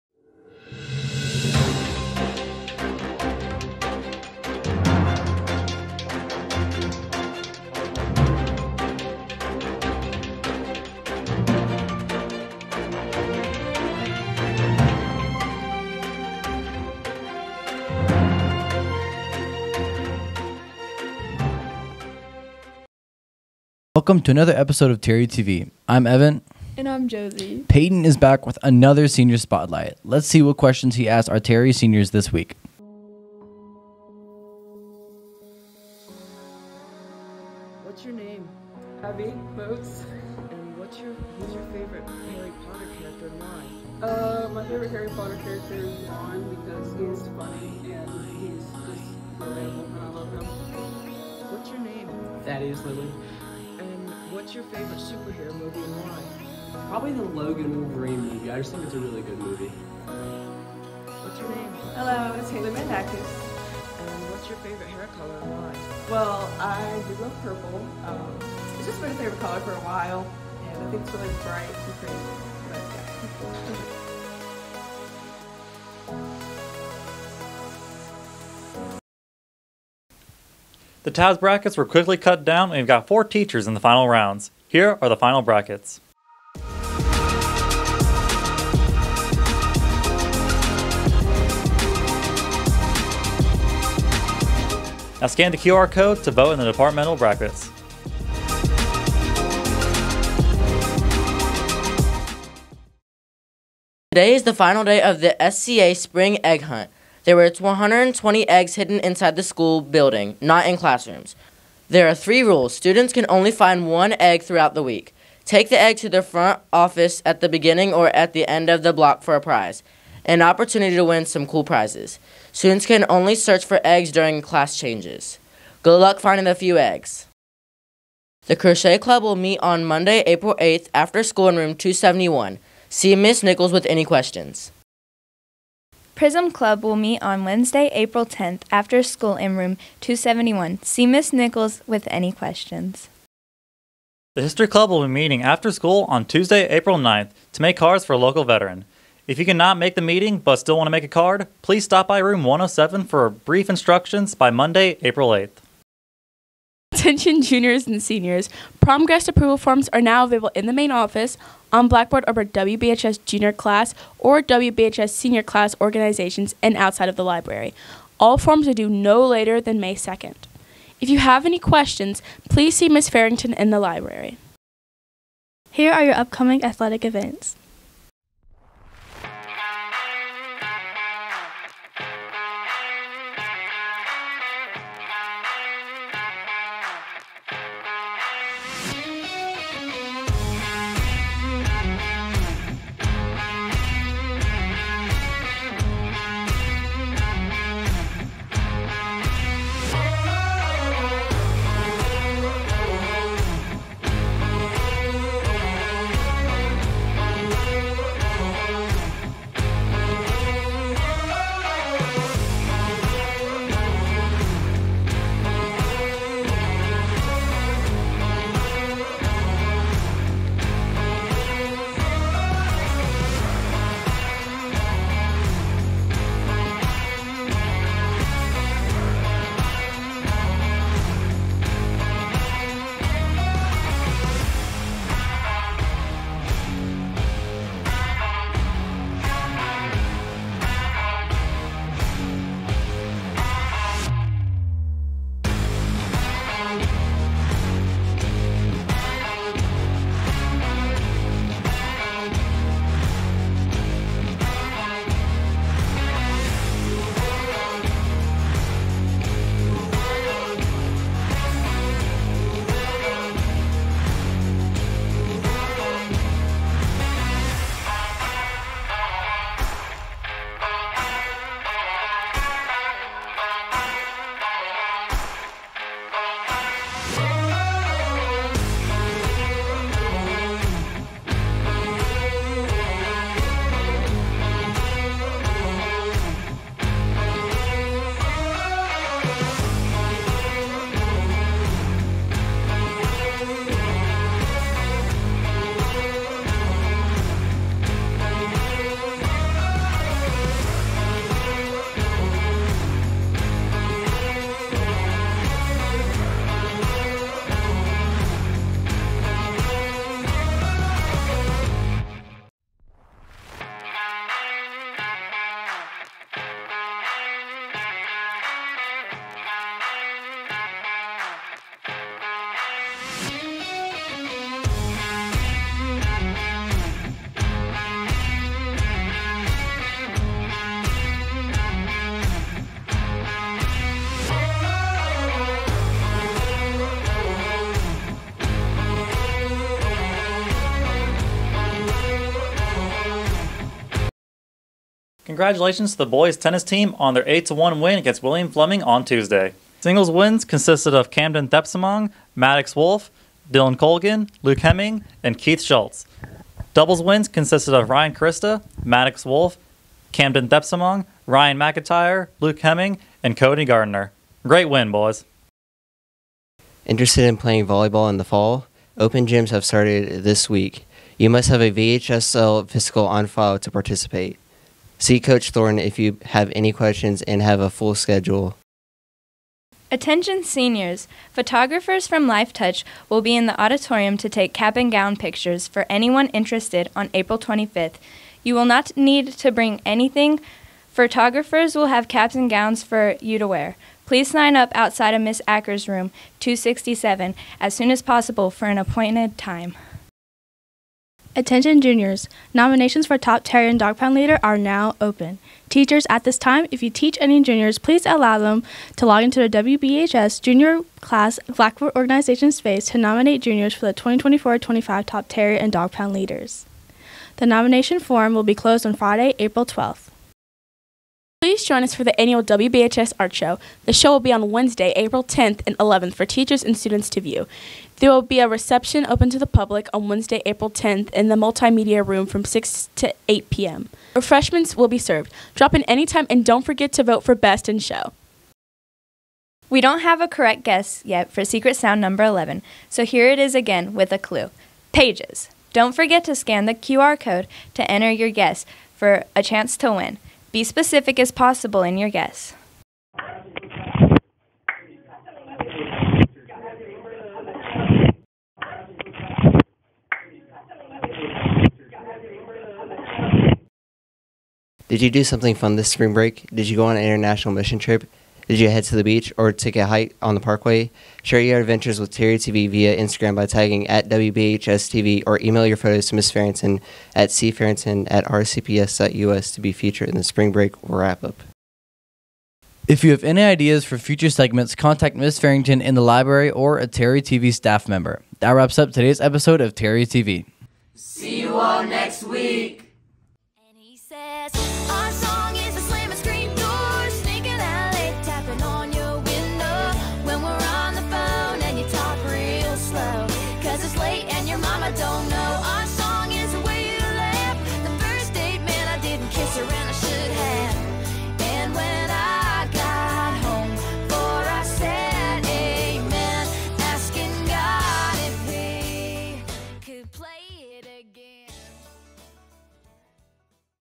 2023-24 Terrier TV Episode 25
In this Friday episode…

- Announcements
- Athletics
- Senior Spotlight
- Secret Sound 11
- T.A.B.S. March Madness Semi-Final Round